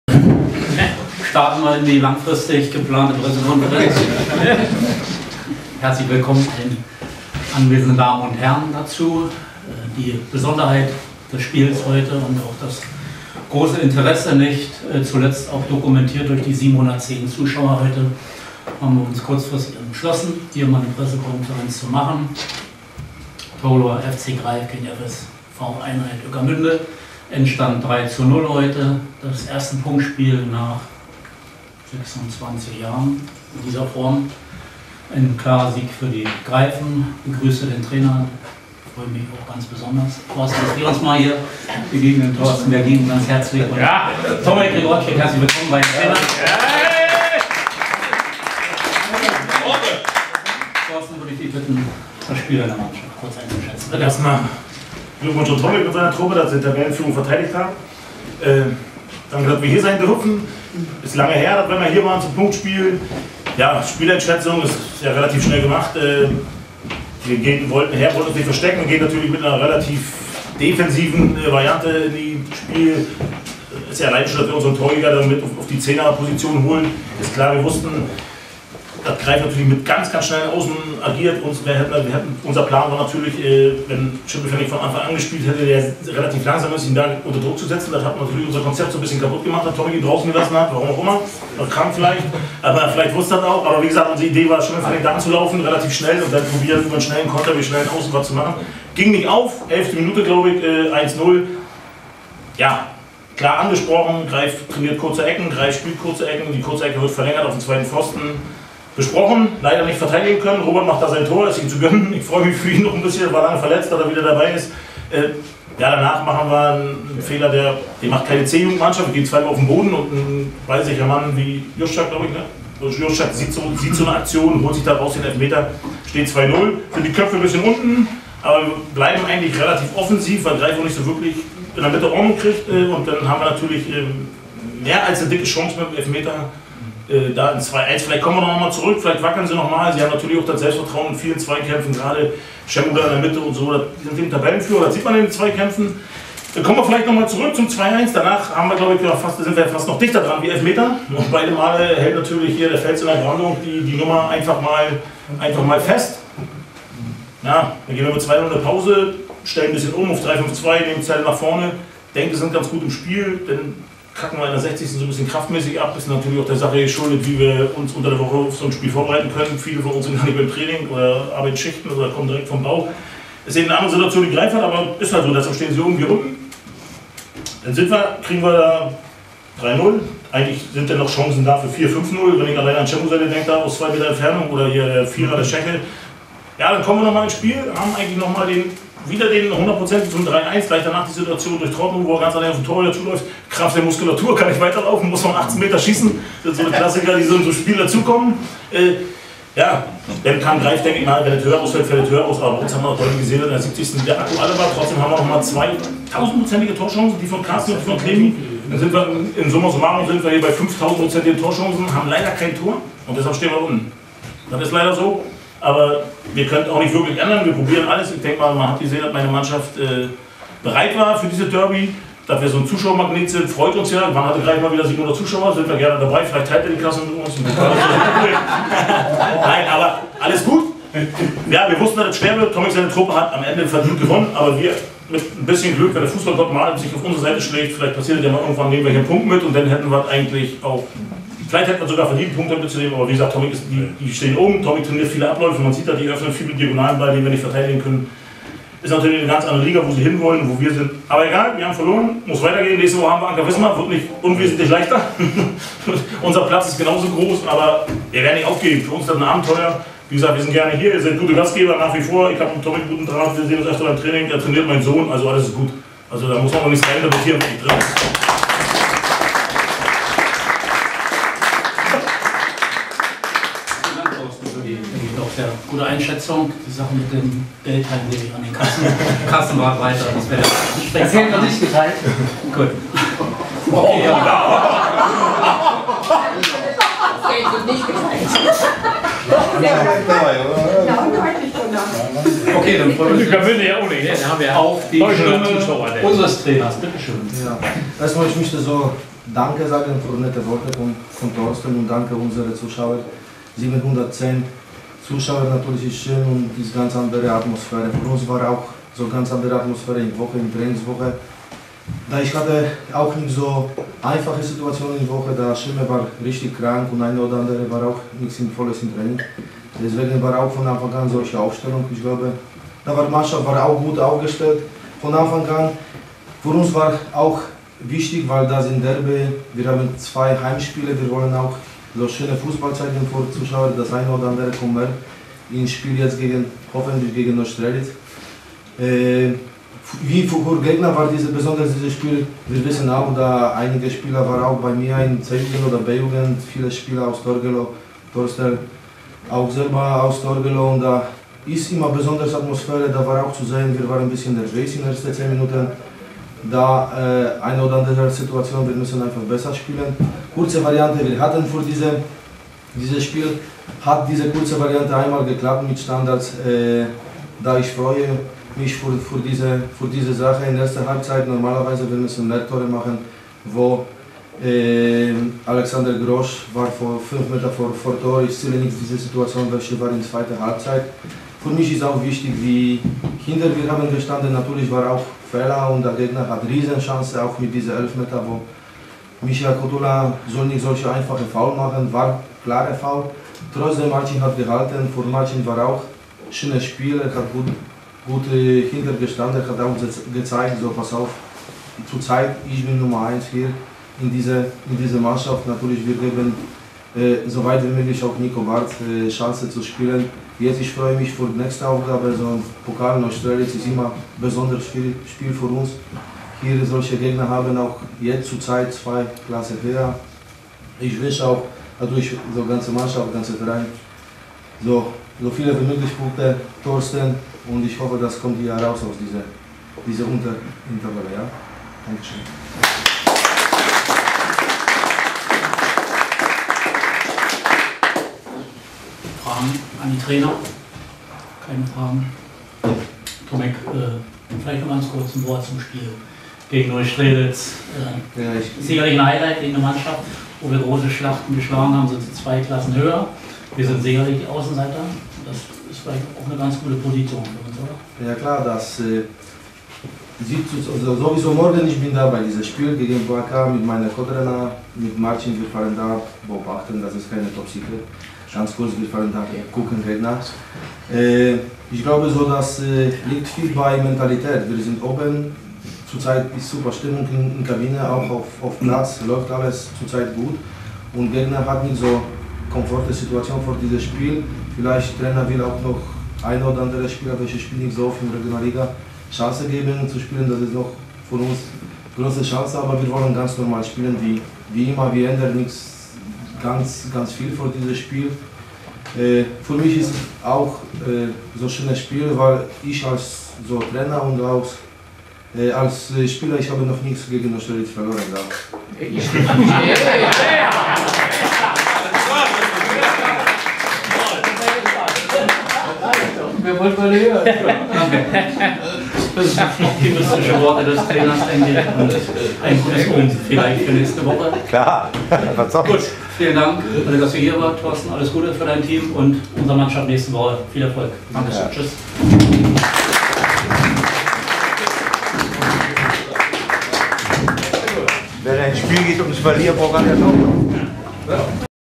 Pressekonferenz 04. Oktober 2014
PK 08.Spieltag - Verbandsliga 2014/15: Torgelower FC Greif vs. FSV 1949 Einheit Ueckermünde 3:0 (2:0)